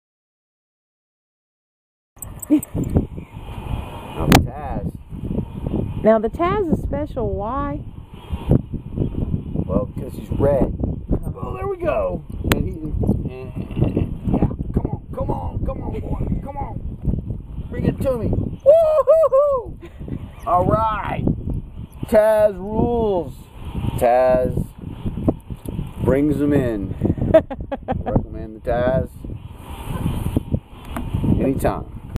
Taz the Bait Slaying Fishing Pole
Taz may be a child's toy fishing pole but it brings in the bait every time.
For more visit: Sandcastlemomma.blogspot.com